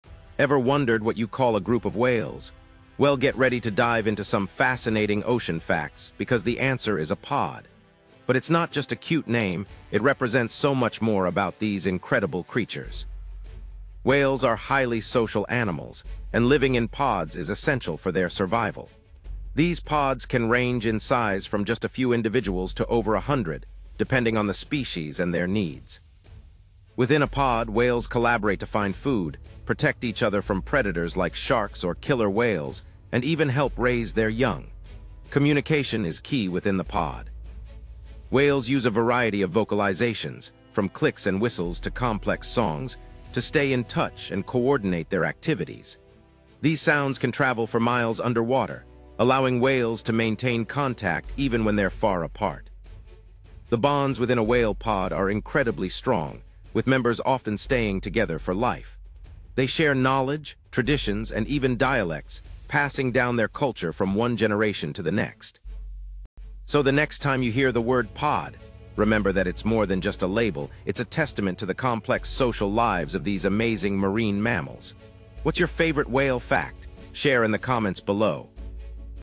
Whale Secrets Revealed!
Did you know a group of whales is called a "pod?" Dive into the fascinating world of whale communication & social structures!